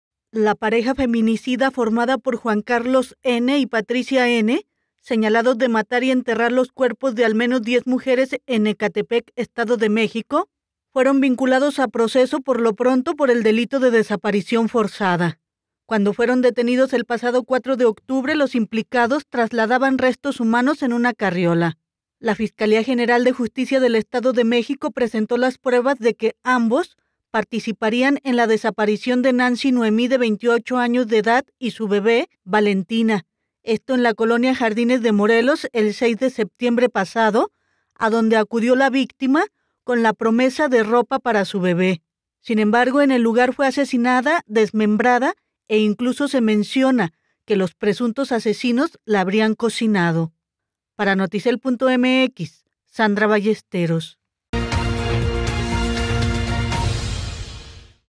A proceso feminicidas
Señalados de haber asesinado y enterrado a un promedio de diez mujeres, la pareja feminicida de Ecatepec, fueron vinculados a proceso por el delito de desaparición forzada